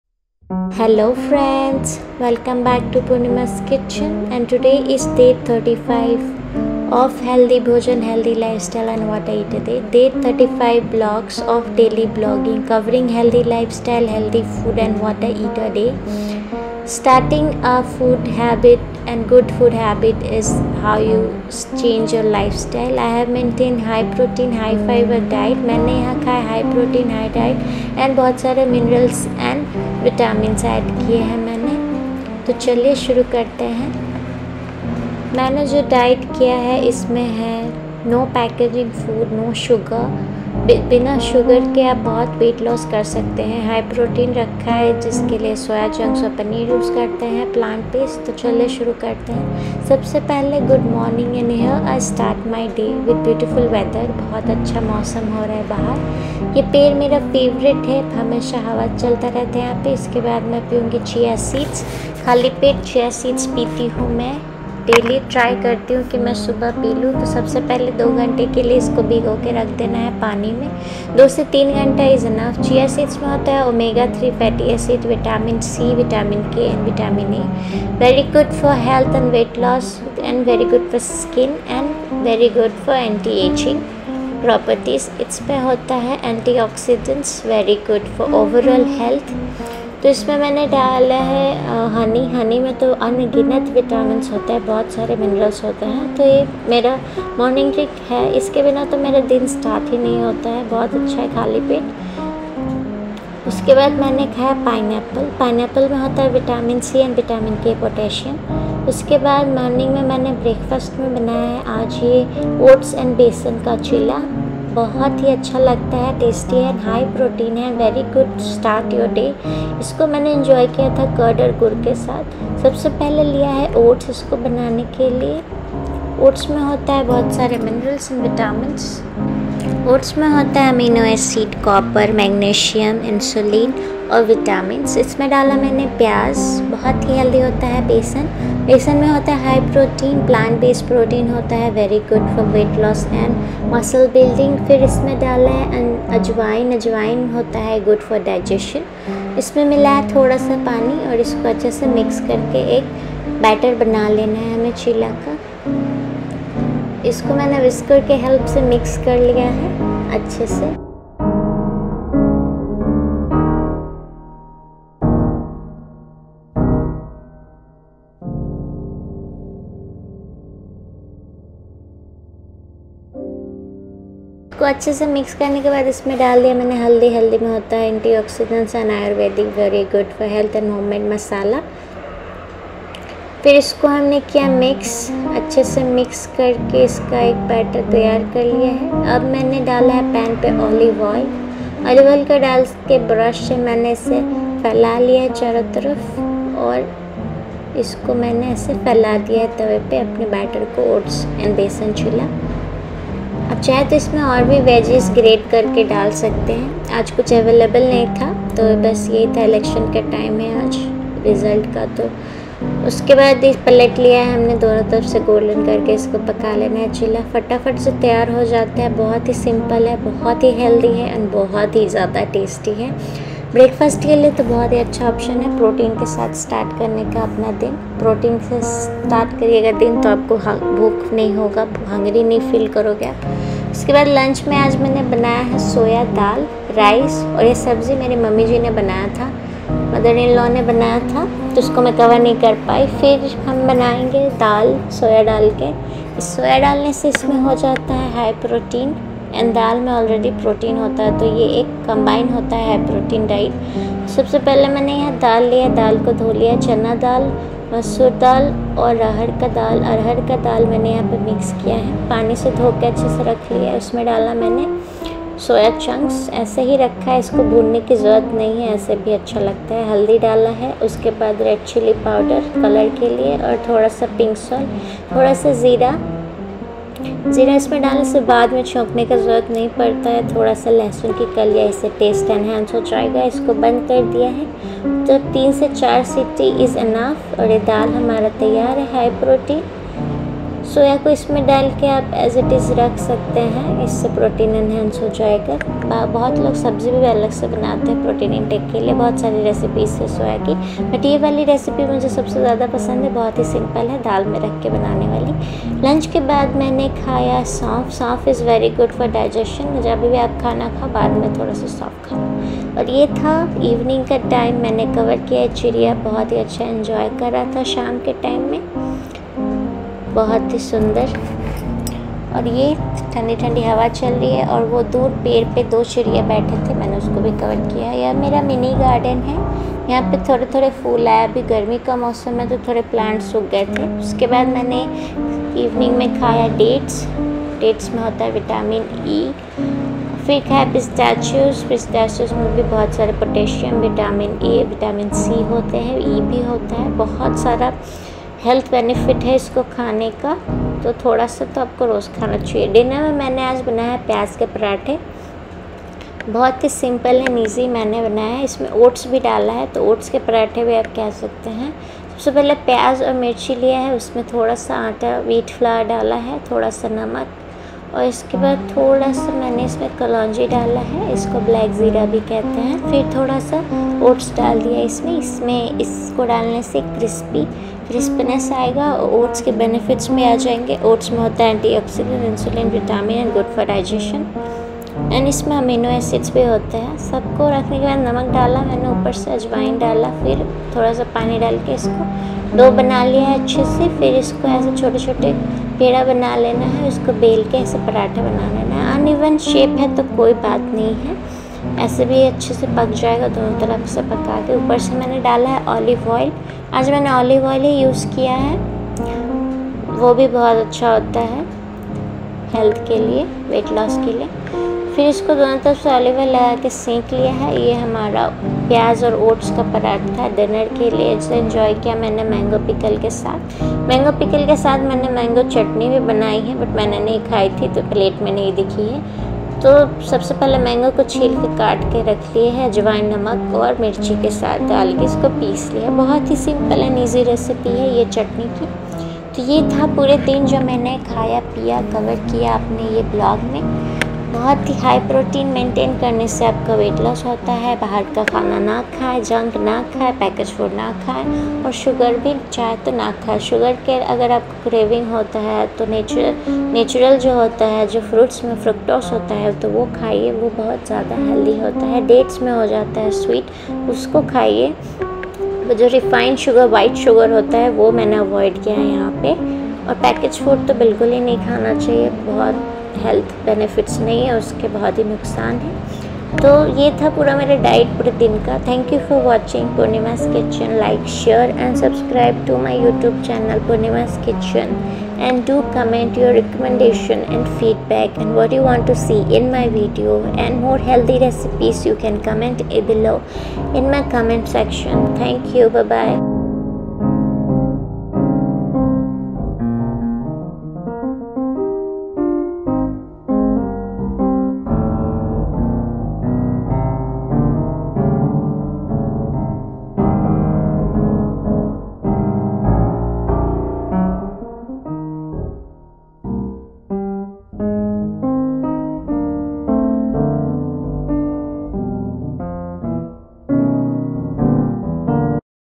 Day 35 Of Healthy Lifestyle💚Daily Vlog | Weight Loss Diet,High Protein & Fibre | Omega -3 Fatty Acid
🌷Hello Friends 🌞Welcome To Purnima'sKitchen🌷Day 35 Vlog🌸
🧚🏻‍♀️Healthy Lifestyle is Healthy Food/Bhojan Habits 🍽️✨
🏋🏻‍♀️10,000 Steps 🚶🏻‍♀️ & Healthy Exercise💪🏼🏋🏻‍♀️& Good Amount of Yoga 🧘🏻‍♀️
🛌Good Amount Sleep💤& 🩵Good Amount of Water Intake 🌊
🍀Veg Healthy Diet 🍀Good For Weight Loss & Healthy Lifestyle🏋🏻‍♀️
🌱Chia Seed is Rich In Omega -3 Fatty Acid & Vitamin C ☘️
❌No Refined Sugar🍬 No Maida🍩No Refined Oil & No package Food🍫
🚫No Tea & No Caffeine Lifestyle 🙅🏻‍♀️{You Can Have 1,2 Cups A Day}☕️*
🌿Pumpkin Seed rich in antioxidants, healthy fats, and minerals🌞
🌻Sunflower Seeds high in vitamin E and selenium & Antioxidants🌻
🌿All Vegetarian Recipes which is Very Good For health & Skin 💃🏻
💪🏼High Protein Food to Built Body And Weight loss {All Veg}🥬
☘️High Fiber Food that is Green Paradise Nature Plant Base🌻
🏋🏻‍♀️High Protein & Fiber, Vitamin C, B12, A ,E & Magnesium, Iron etc. ❄️

👇🏼To Watch my All Vlogs Click Below Links👇🏼